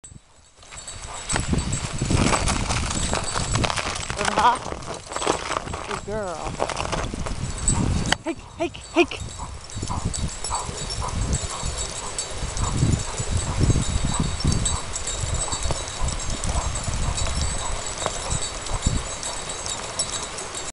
Running in the snow
A glimpse of our run in the snow today. Keeping it short because it takes to long to upload. Best part about running in the snow is both dog and sulky come home clean, lol!